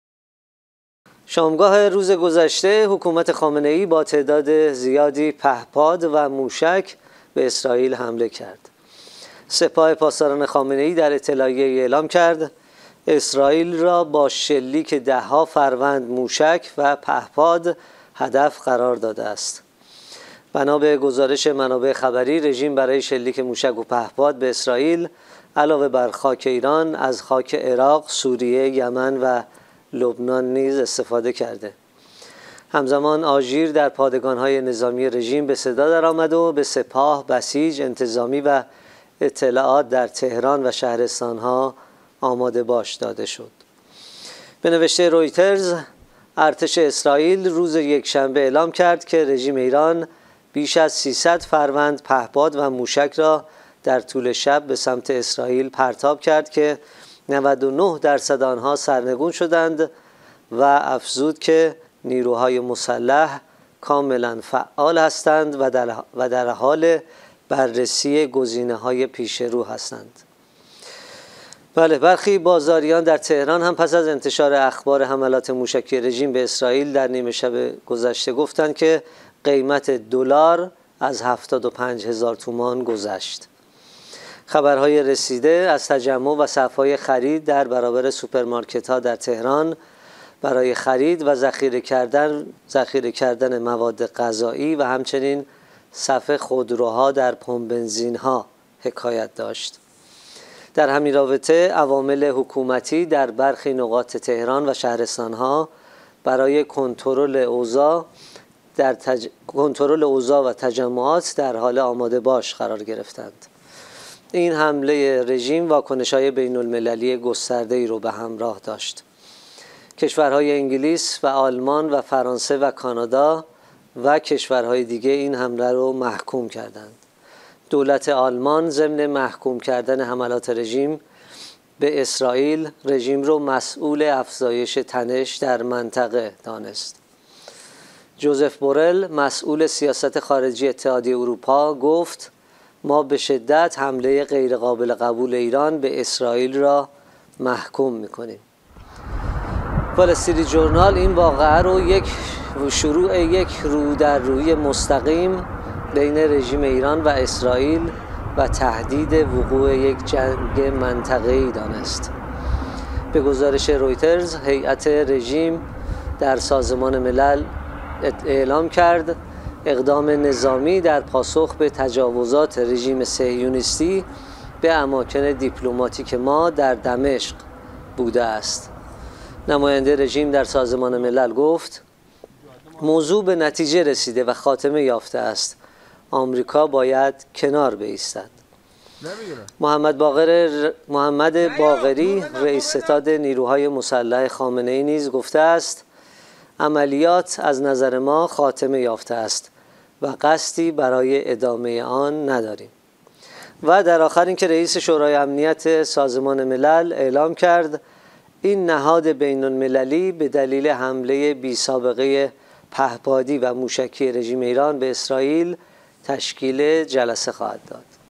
حمله پهپادی و موشکی رژیم به اسرائیل
🔴 گزارش روز...
به نوشته رویترز: ارتش اسرائیل روز یکشنبه اعلام کرد که (رژیم) ایران بیش از ۳۰۰ فروند پهپاد و موشک را در طول شب به سمت اسرائیل پرتاب کرد که ۹۹ درصد آنها سرنگون شدند